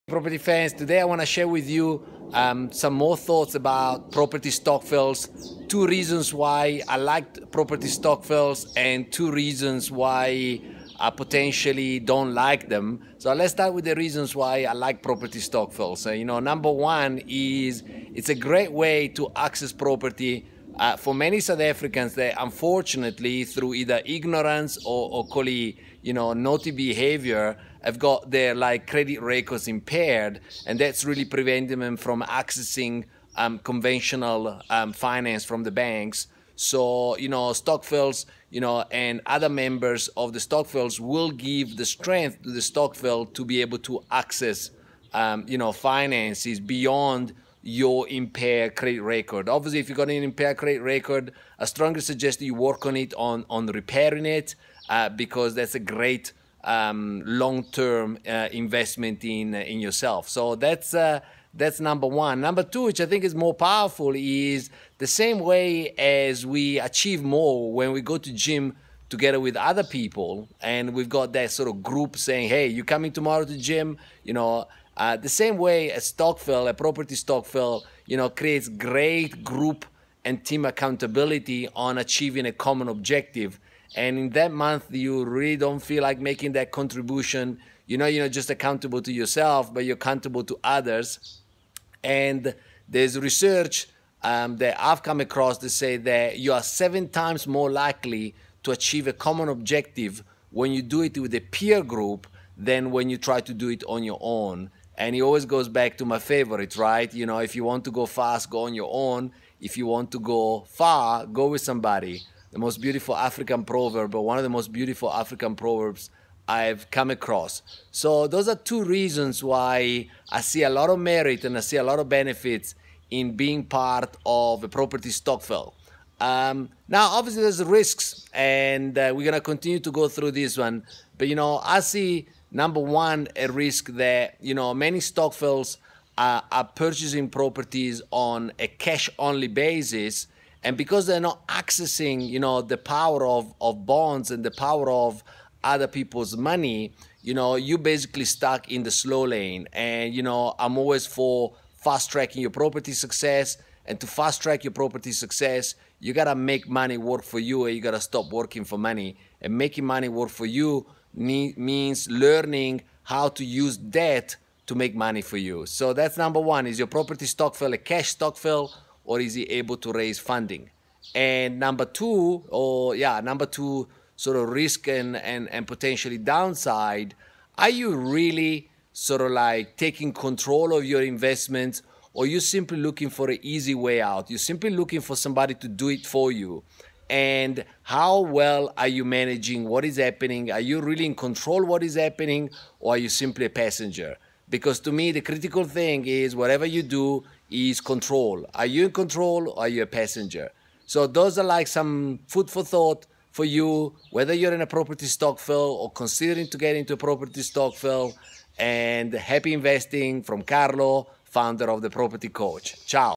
Property Stokvels - yes or no? Episode 2
Pros and Risks of doing Property via Stokvels